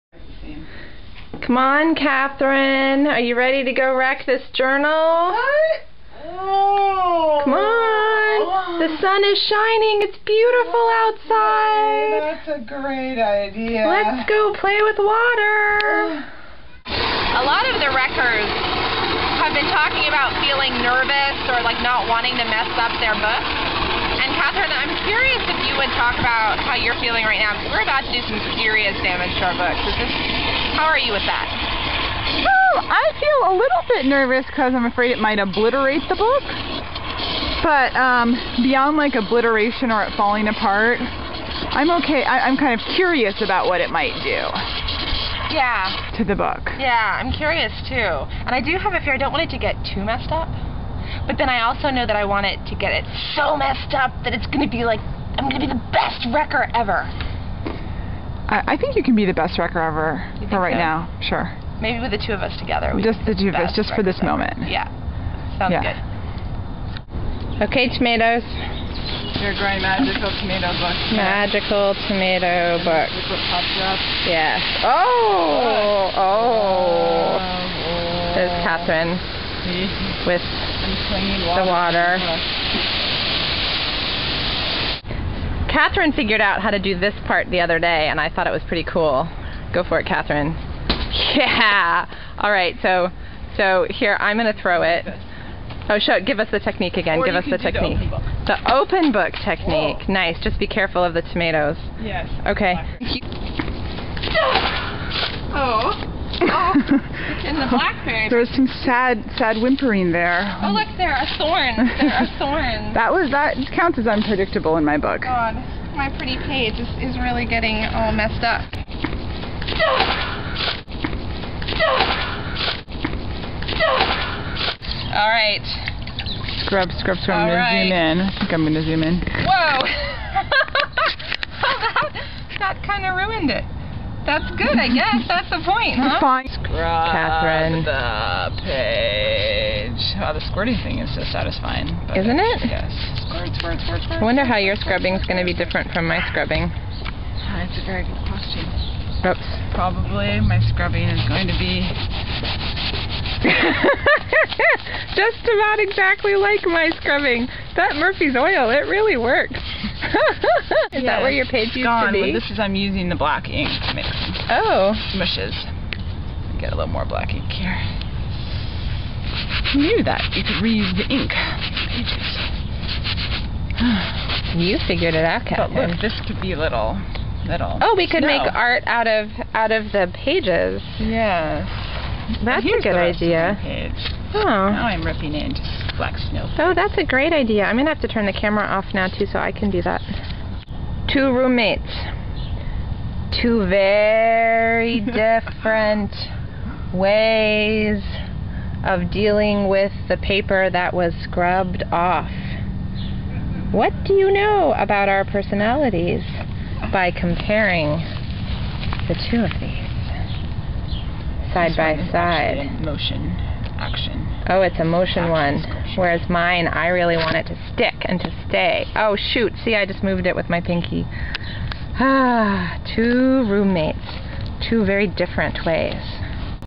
Wreck This Journal Week 5
Wreck This Journal! Katherine is exhausted at 6pm on Friday after a full week's work. But roommate Gretchen convinces her to do some joint wreckage...